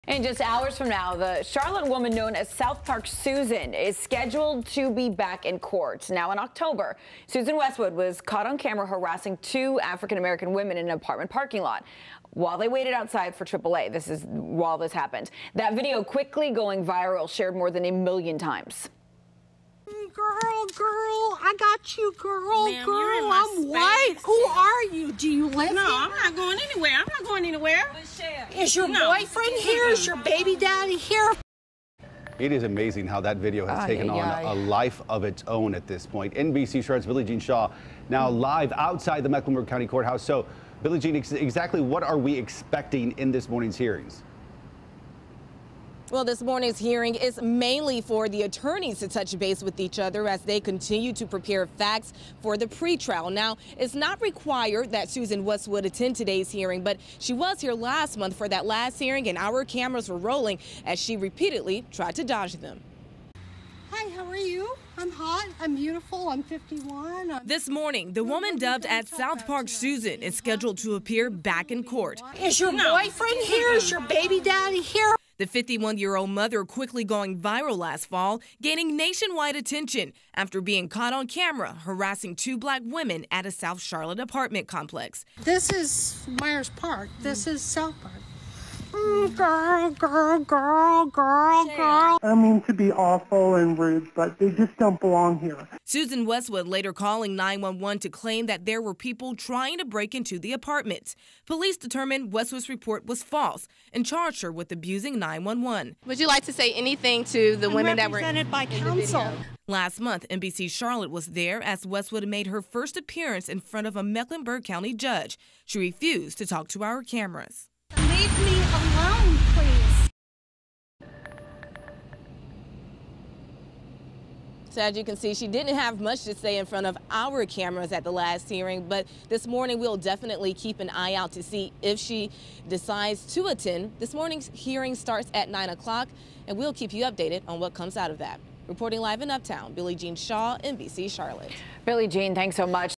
'South Park Susan' expected in court Wednesday
The Charlotte woman known as "South Park Susan" is expected to appear in court Wednesday. Susan Westwood went viral last October when she was caught on camera harassing two black women in an apartment parking lot.